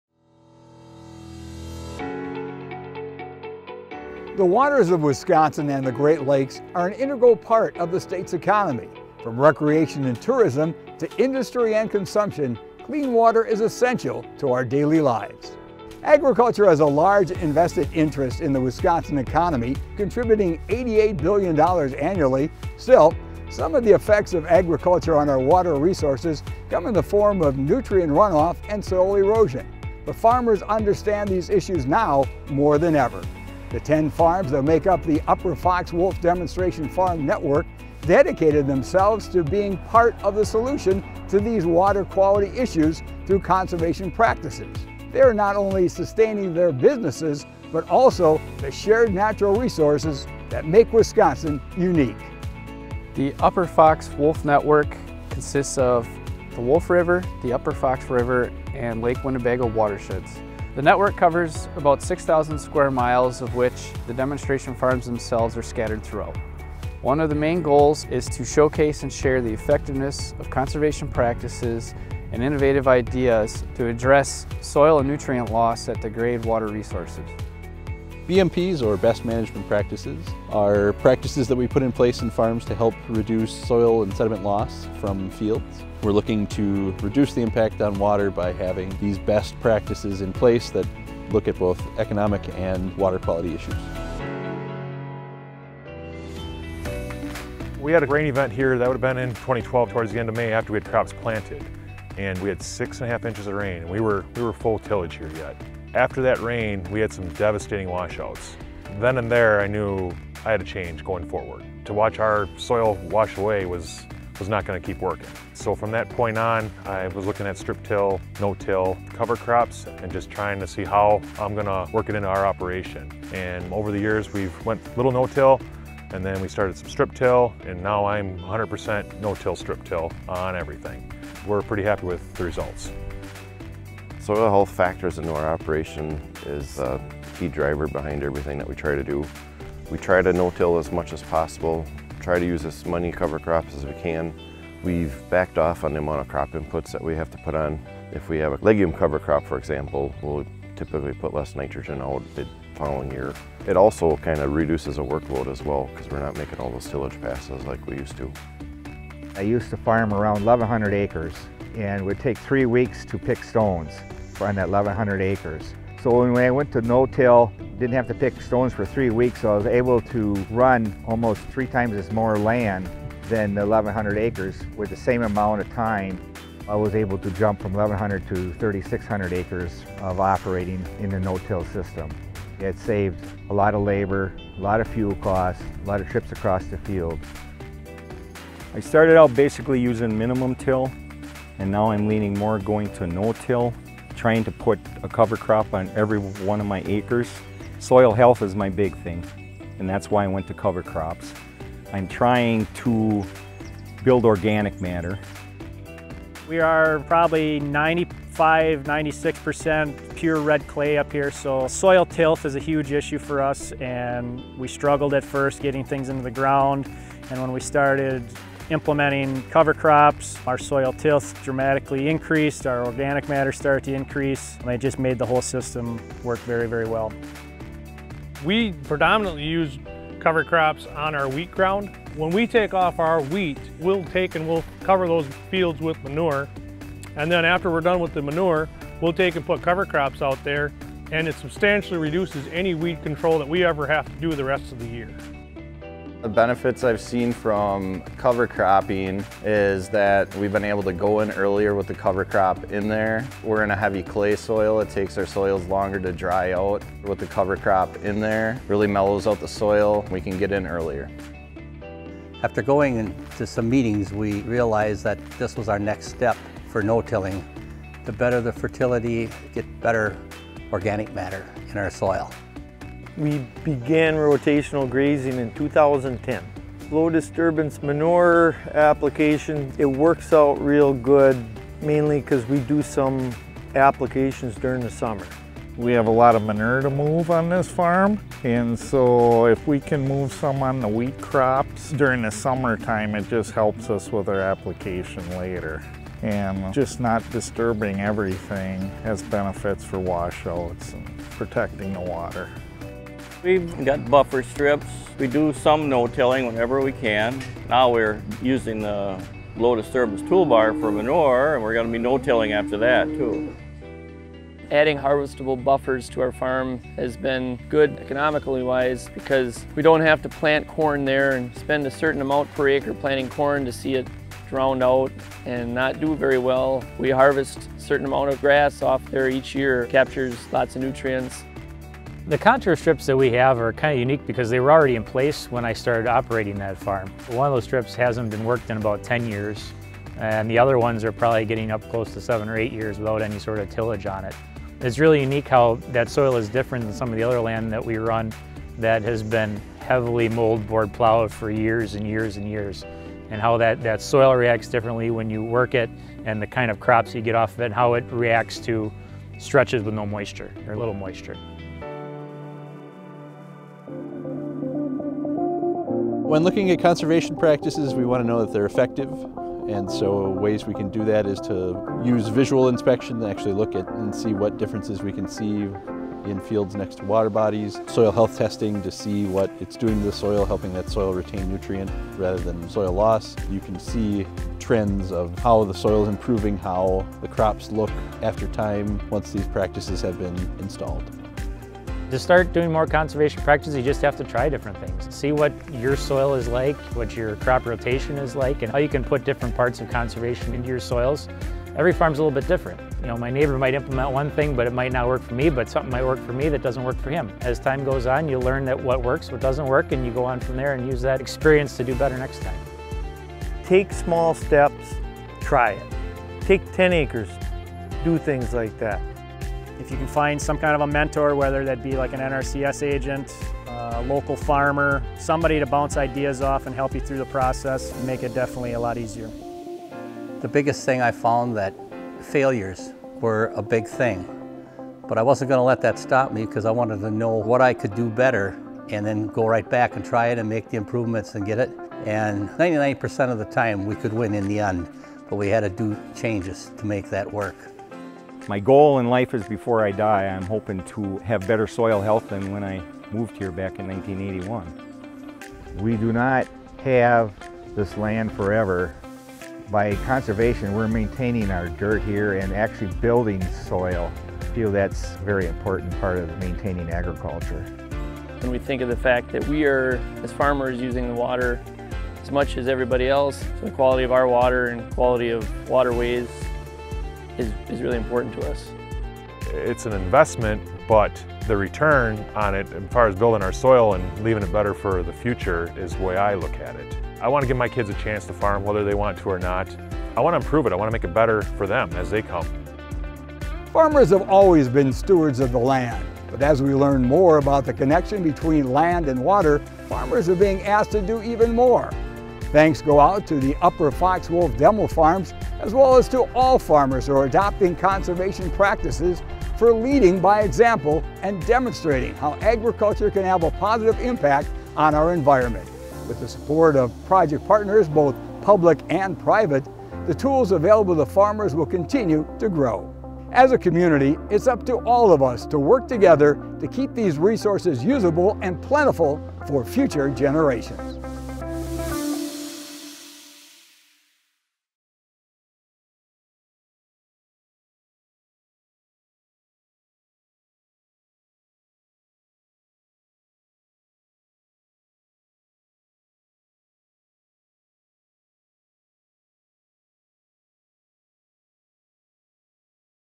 Upper Fox-Wolf Demonstration Farms Network
Members of the Upper Fox-Wolf Demonstration Farms Network are working to restore our natural resources through conservation, so future generations can continue the tradition of sustainable agriculture in Wisconsin. The Demo Farms is a Great Lakes Restoration Initiative project designed to showcase and demonstrate leading edge conservation practices that improve Great Lakes water quality by reducing phosphorus from entering Green Bay and Lake Michigan.

The USDA Natural Resources Conservation Service and Waupaca County Land & Water entered into an agreement to establish the network, in partnership with Fond du Lac, Green Lake, Marquette, Outagamie, Portage, Shawano, and Winnebago counties and the Green Lake Association. Video design by Dan Hagenow, @DanHagenowVideoCreation.